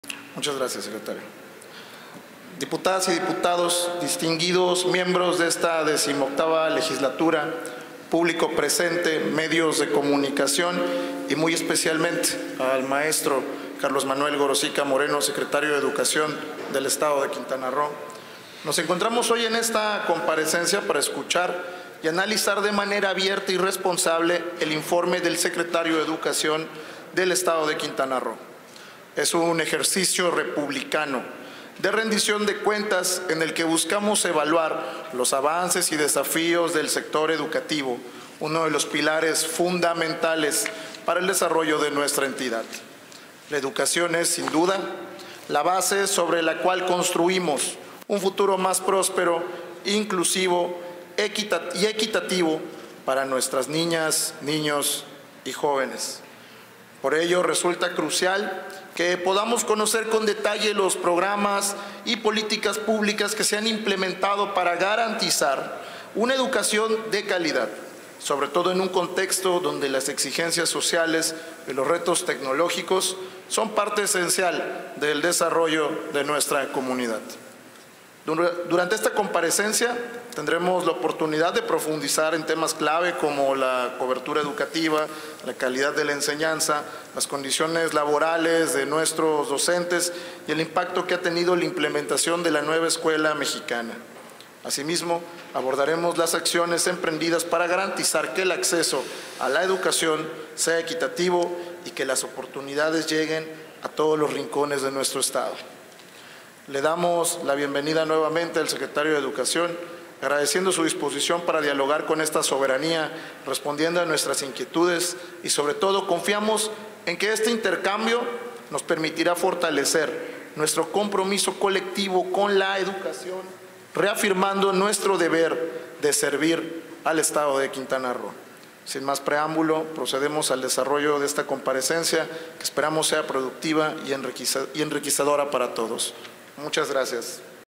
Intervención del Dip. Saulo Aguilar Bernés. Glosa 2do Informe (04/Octubre/2024).
Palabras de bienvenida del Dip. Saulo Aguilar Bernés, Presidente de la Comisión de Educación, Ciencia y Tecnología de la H.XVIII Legislatura, en la Comparecencia del Mtro. Carlos Manuel Gorocica Moreno, Secretario de Educación del Estado de Quintana Roo, para la  para la Glosa del 2do Informe de Gobierno.